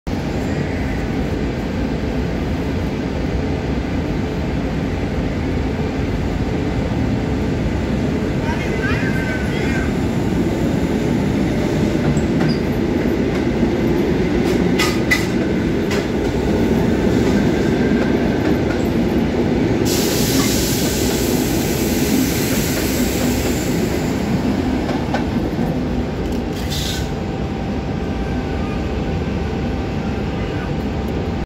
MR79 773+MR78 744+MRCR 986 vertrekken uit Brussel-Noord als IC-11 3435 naar Binche
IC-11 3435 naar Binche met 1x MR79-treinstel van 773, 1x MR78-treinstel van 744 en 1x MRCR-treinstel van 986.
Deze trein vertrok uit Brussel-Noord op spoor 10 om 14:18.
(Niet gemaakt door mij)
Deze video is gemaakt op 18/09/2022.